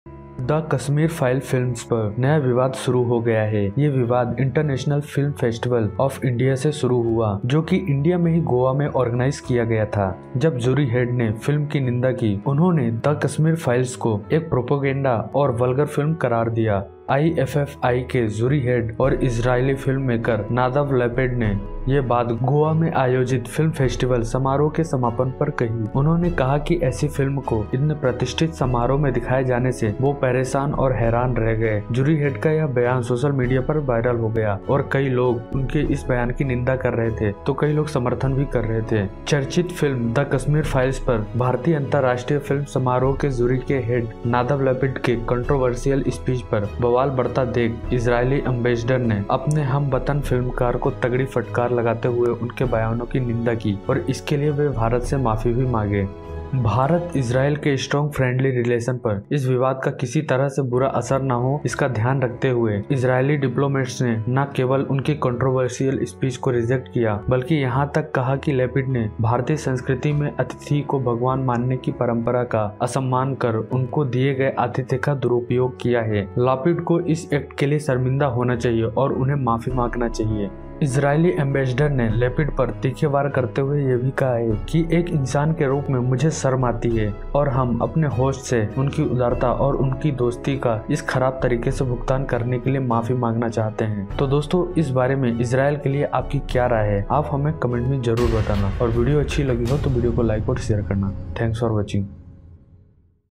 the kashmir files controversy , Israel ने भारत से मांगी माफी
_____'द कश्मीर फाइल्स' फिल्म पर नया विवाद शुरू हो गया है. ये विवाद IFFI “ इंटरनेशनल फिल्म फेस्टिवल ऑफ इंडिया “  से शुरू हुआ जो की इंडिया मे ही गोवा में organise किया गया था , जब जूरी हेड ने फिल्म की निंदा की. उन्होंने 'द कश्मीर फाइल्स' को एक प्रोपेगेंडा और वल्गर फिल्म करार दिया है. IFFI के जूरी हेड और इजरायली फिल्ममेकर नादव लैपिड ने ये बात गोवा में आयोजित फिल्म फेस्टिवल समारोह के समापन पर कही. उन्होंने कहा कि ऐसी फिल्म को इतने प्रतिष्ठित समारोह में दिखाए जाने से वो परेशान और हैरान रह गए.
जूरी हेड का यह बयान सोशल मीडिया में वायरल हो गया और कई लोग उनके इस बयान की निंदा कर रहे हैं तो कोई समर्थन कर रहा है.

चर्चित फिल्म 'द कश्मीर फाइल्स' पर भारतीय अंतर्राष्ट्रीय फिल्म समारोह की जूरी के Head नादव लापिड के Controversial speech पर बवाल बढ़ता देख ……इजरायली Ambessdor ने अपने हम वतन फिल्मकार को तगड़ी फटकार लगाते हुए उनके बयानों की निंदा की और इसके लिए भारत से माफी भी मांगी।
 भारत-इजराइल के strong Friendly relation पर इस विवाद का किसी तरह से बुरा  असर न हो इसको ध्यान रखते हुए  इजरायली Diplomates ने न केवल उनके controversial speech को reject किया बल्कि यहां तक कहा कि लापिड ने भारतीय संस्कृति में अतिथि को भगवान मानने की परंपरा का असम्मान कर , उनको दिए गए आतिथ्य का दुरूपयोग किया।______

 @Ago world